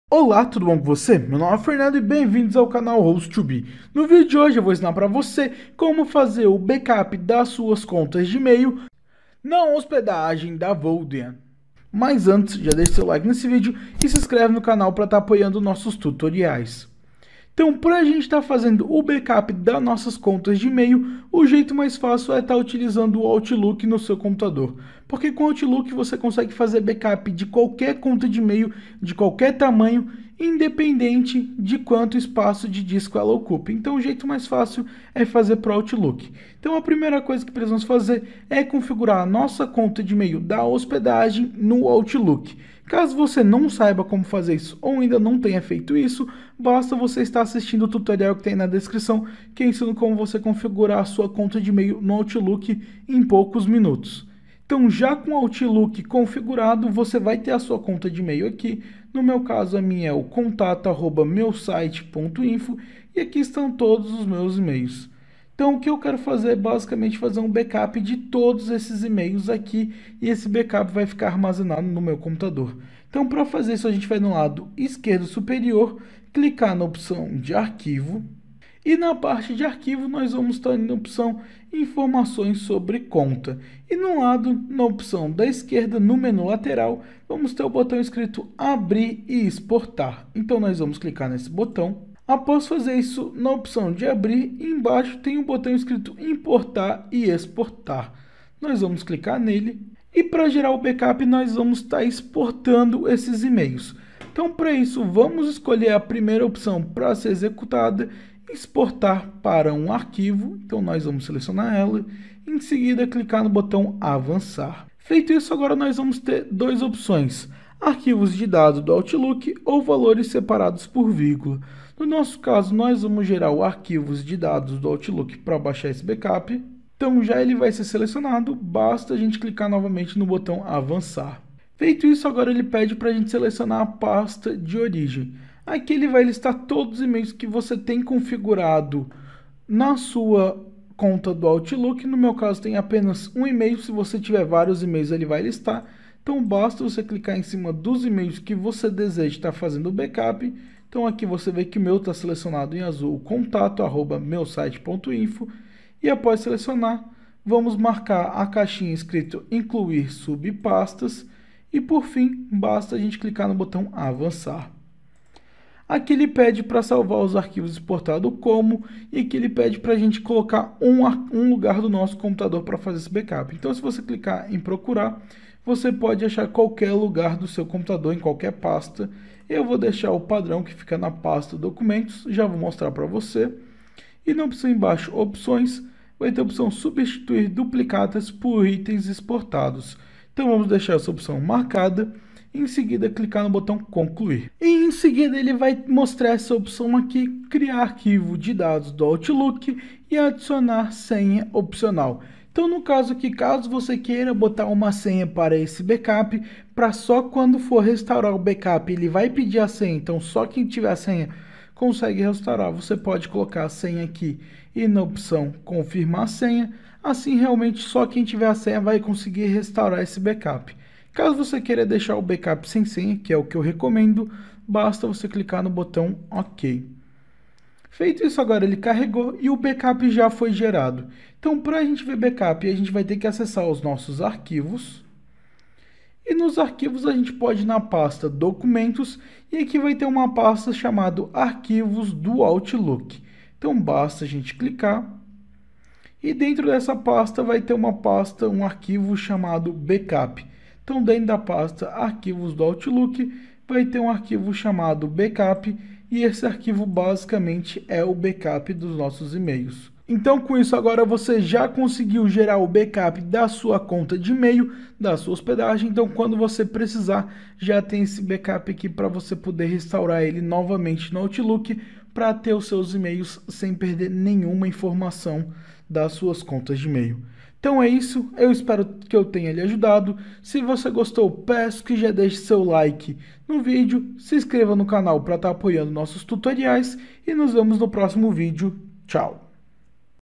Como Fazer Backup dos Emails na Vodien – rápido e fácil – 2022
Aprenda Como Fazer Backup dos Emails Na Vodien facilmente e em poucos minutos